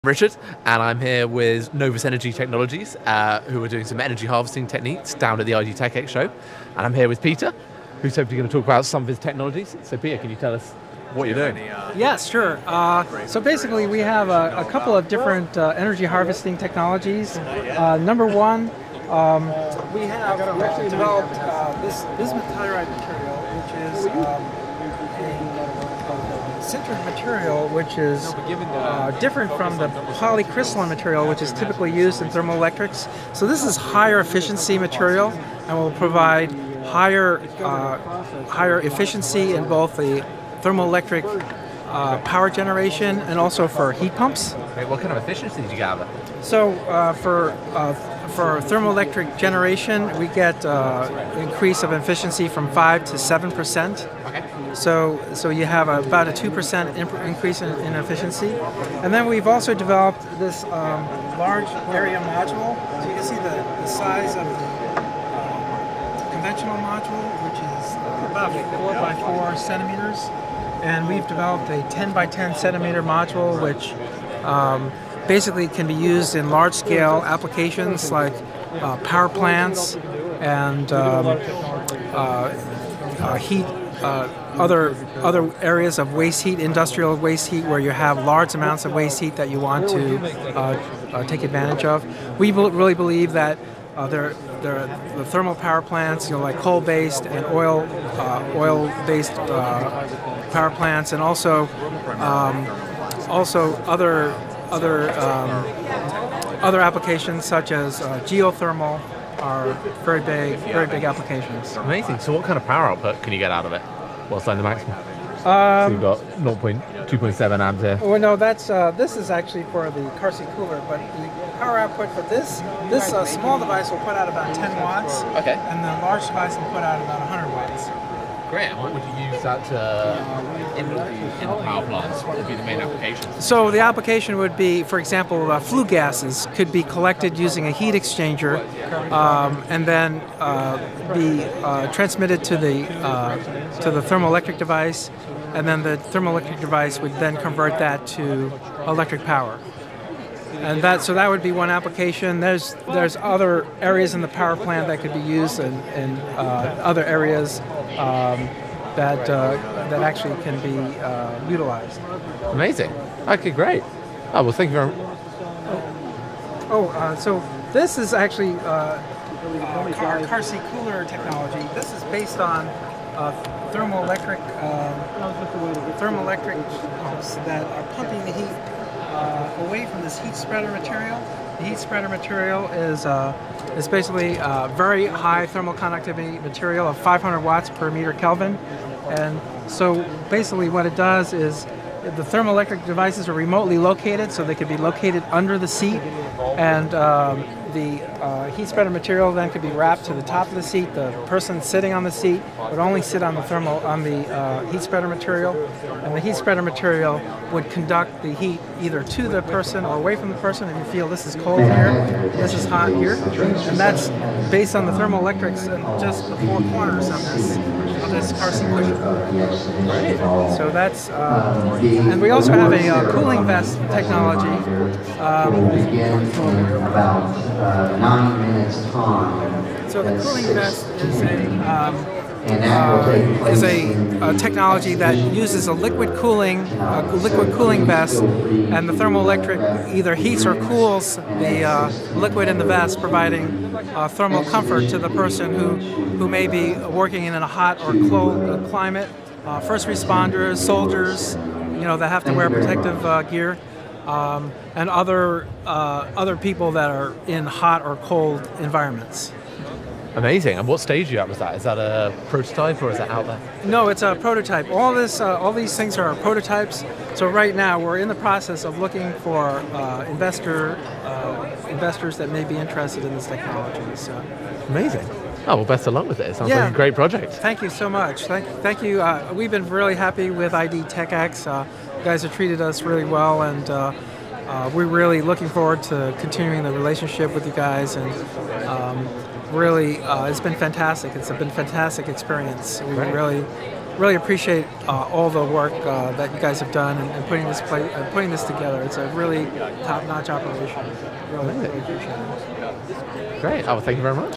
Novus Energy shows solid state cooling/heating and heat-to-electric conversion technology
is a rapidly growing market disrupter globally focused on next generation solid state cooling/heating and heat-to-electric conversion technology (currently $500M market growing 30%-CAGR). Novus'products include low cost, large area XL-TEGenerator and XL-TECH-CoolingHeating modules, high temperature HT-TEG modules, MaxTherm automobile seat cooler/heater and i-Therm personal cooling/heating vest. Filmed at the USA 2016 tradeshow in Santa Clara California.